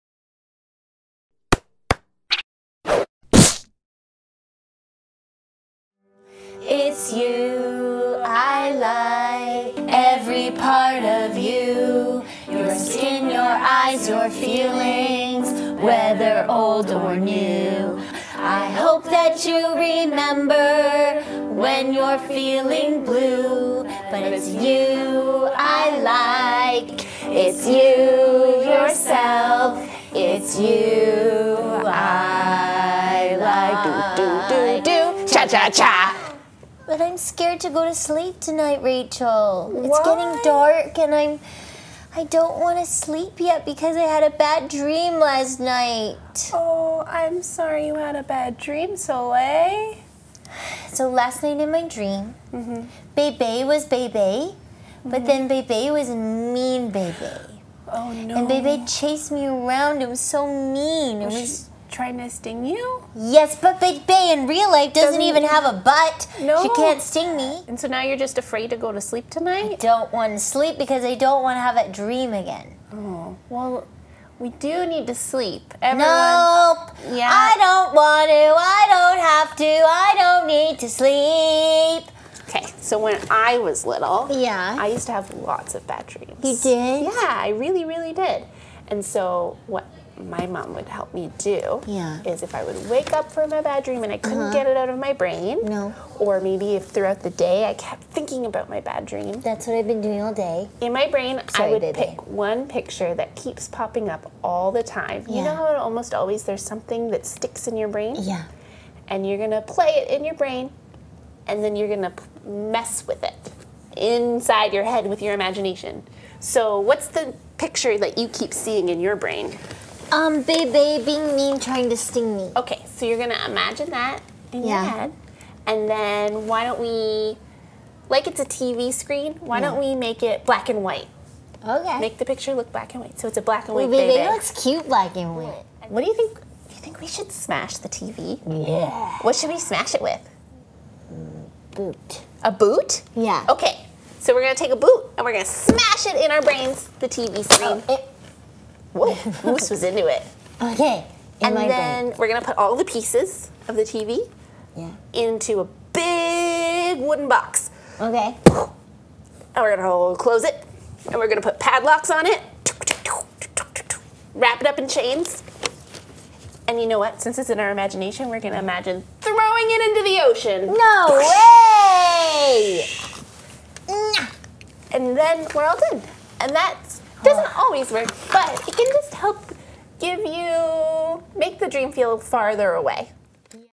The Rainbow Show: Coping with Bad Dreams
In this edition: Child life specialist, Rachael, and SickKids' Therapeutic clown, Soleil, talk about bad dreams and how to feel better